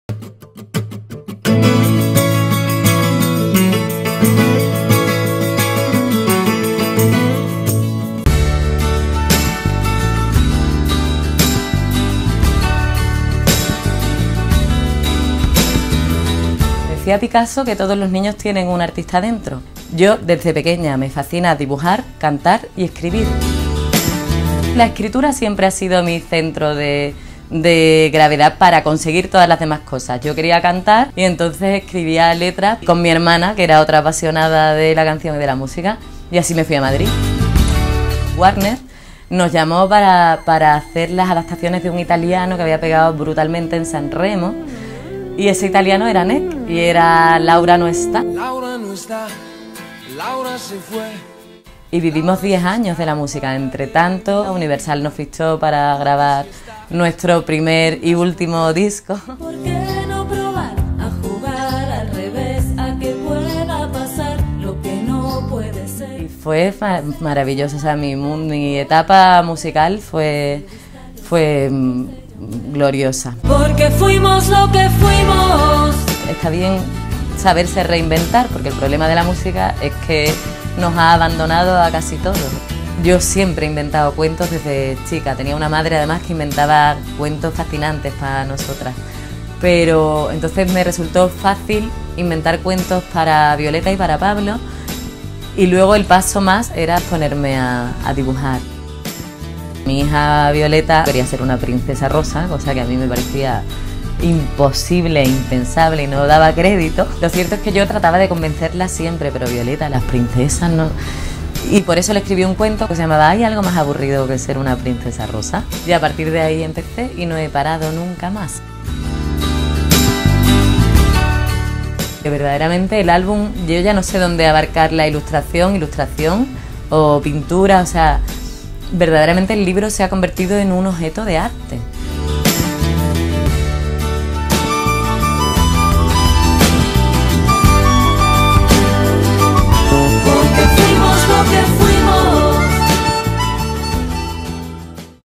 NA Raquel Díaz Reguera, l’artiste qui catalogue les sentiments avec des illustrations
Écrire, chanter et dessiner. Et pas spécialement dans cet ordre. Ce sont les trois piliers de la vie artistique de cette andalouse de Séville Raquel Díaz Reguera (1974), qui lorsqu’elle s’est vue ‘abandonnée’ par la musique et ‘acceptée’ par la maternité, a découvert un monde fascinant : celui des albums illustrés. Elle avait passé plus d’une décennie dédiée à la musique comme parolière et d’un disque à succès avec sa sœur Nuria avec laquelle elle avait formé le groupe Maldeamores (2000). Son premier album d’illustrations fut un essai pour persuader sa fille Violeta d’abandonner l’idée de vouloir être une princesse. Quatre ans plus tard, ‘Y-a-t’il quelque chose de plus ennuyeux que d’être une princesse rose?’ (2010) en est à sa sixième édition, a été traduit en trois langues et s’est converti en matériel de référence dans la coéducation. Cela fut seulement le début. Entre 2010 et 2014 elle a publié 22 titres et son esprit est constamment en ébullition. Elle adore inventorier les choses et les célébrités avec ses catalogues de mères, grand-mères et baisers. Le dernier, ‘Amour et c’est tout’. Parce que les points de suspension marquent la cadence de sa vie.